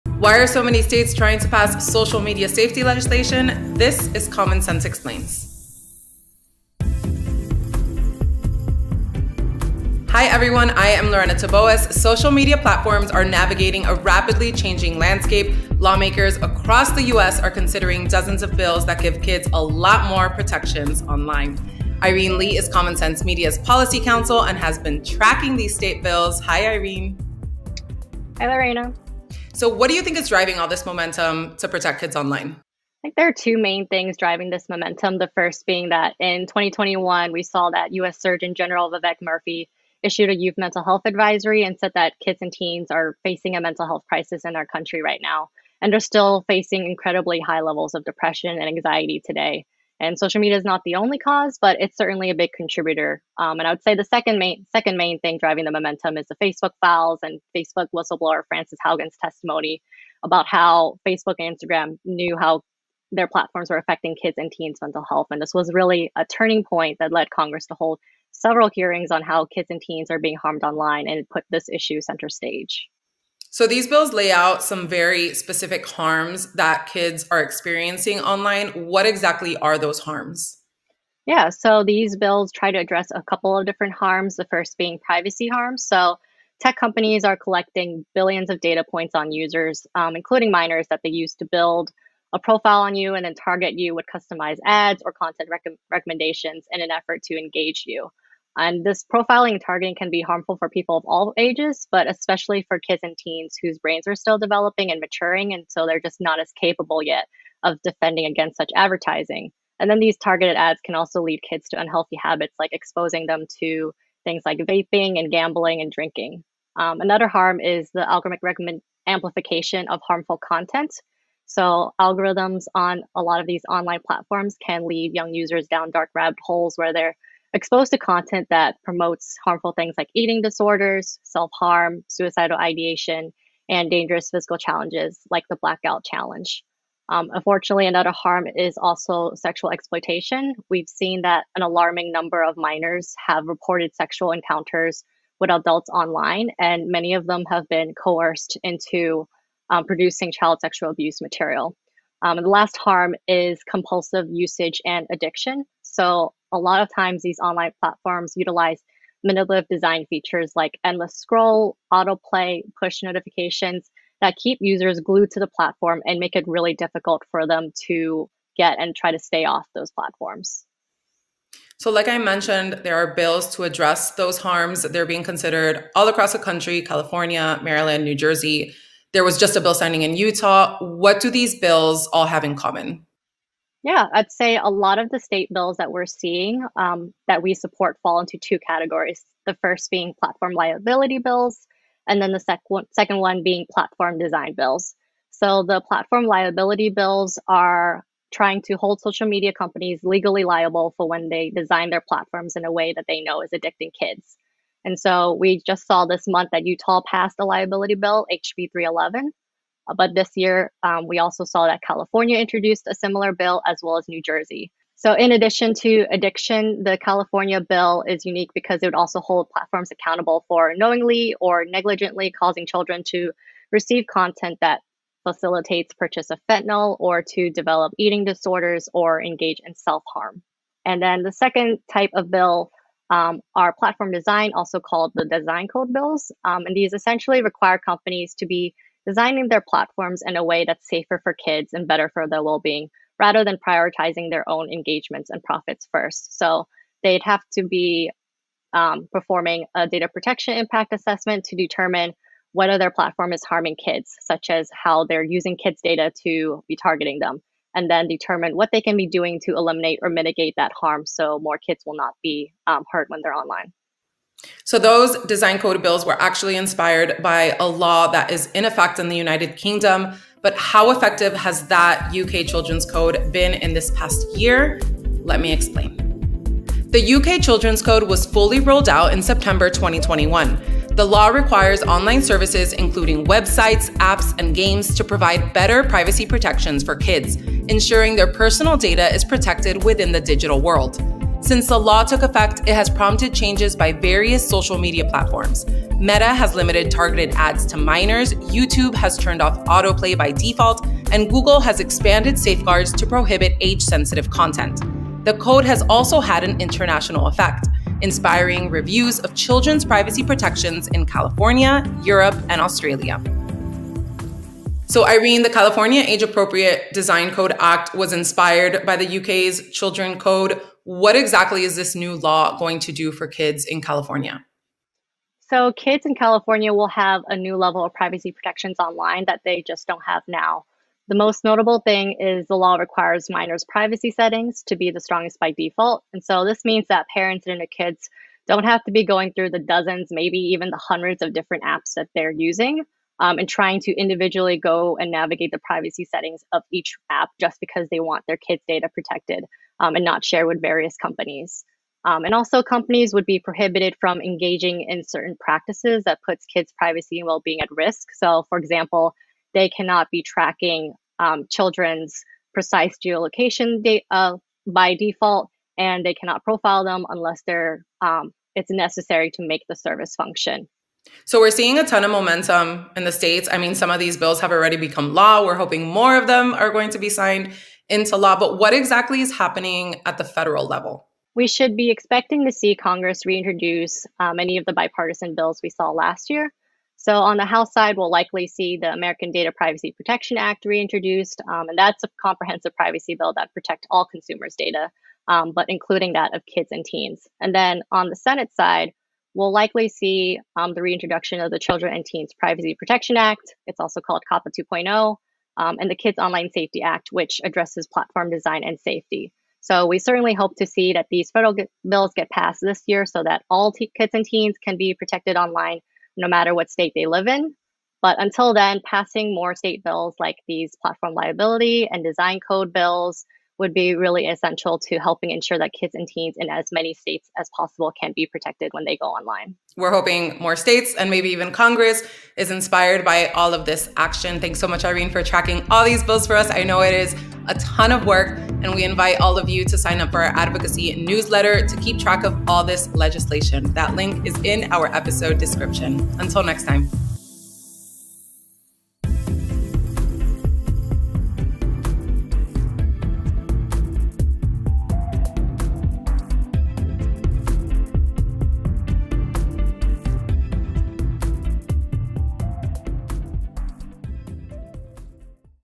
Are states (finally) stepping up for kids' safety online? | Common Sense Explains
Our Stress-Free App for Finding The Best Kids’ Entertainment In this episode, Policy Counsel Irene Ly explains why there's so much momentum right now at the state level to protect kids' online. You'll learn about how these bills are focused on both holding platforms accountable for harms they're causing to kids and to designing platforms with kids' safety in mind. You'll hear about how Maryland and California are building off of reforms passed in the United Kingdom that are already impacting how social media platforms behave, and you'll also hear how the federal government is responding.

Sign up for our advocacy newsletter to get updates on how you can support kids' safety and check out our blog for more insights into media, tech, and kids